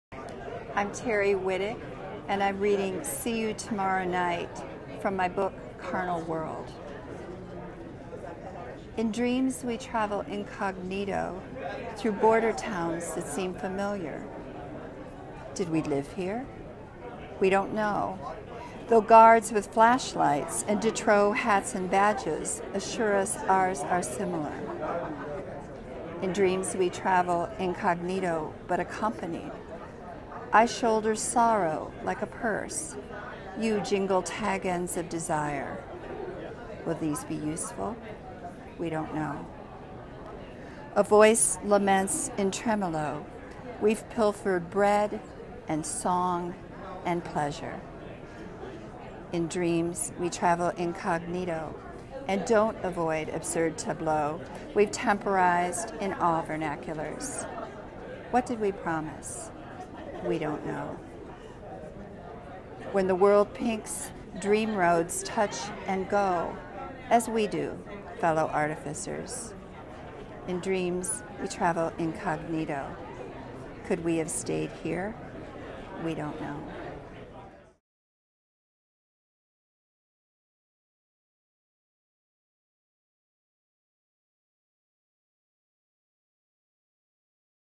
Weekly Poem: 'See You Tomorrow Night' by Terri Witek
Terri Witek is the Art & Melissa Sullivan Chair in Creative Writing at Stetson University. She is the author of "The Shipwreck Dress," (2008, Florida Book Award Winner), "Carnal World" (2006), "Fools and Crows" (2003), and "Courting Couples" (2000 Center for Book Arts Prize).